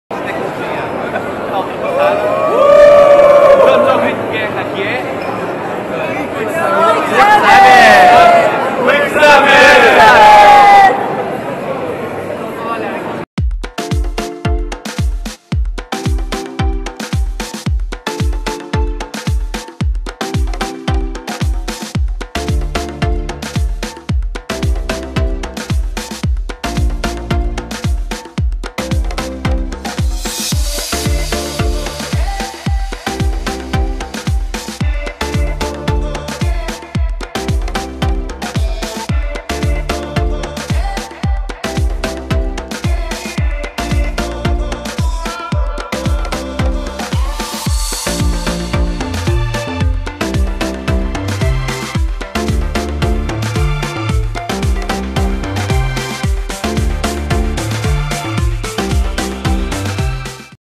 Quem está neste vídeo 360 no ClickSummit?
Quem está neste vídeo 360? Foi captado na última edição do ClickSummit. Juntei mais algumas memórias deste evento que tenho acompanhado desde 2014, quando começou. Para quem quer atualizar-se, aprender mais e fazer networking, é um evento de qualidade.

Vou lá estar nos dias 11 e 12 de outubro, ainda podem reservar o vosso lugar